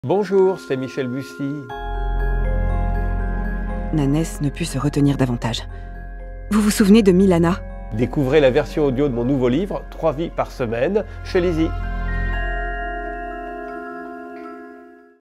LIZZIE PRÉSENTE... Trois vies par semaine, de Michel Bussi
Michel Bussi vous présente son nouveau roman "Trois vies par semaine" à découvrir en livre audio chez Lizzie !

Un corps est retrouvé dans la vallée de la Meuse, au coeur des Ardennes. Accident, suicide, meurtre ? La révélation de l'identité de la victime plonge la capitaine Katel Marelle dans la sidération. Renaud Duval menait-il... trois vies par semaine ? Trois femmes attendent son retour. Chacune revendique d'être son unique amour. Ensemble, elles vont tenter de percer l'énigme d'une impossible triple vie... Mais comme dans un théâtre d'illusions, des ombres rôdent, prêtes à se venger. Qui sait la vérité ? Qui manipule ? Qui tire les ficelles ?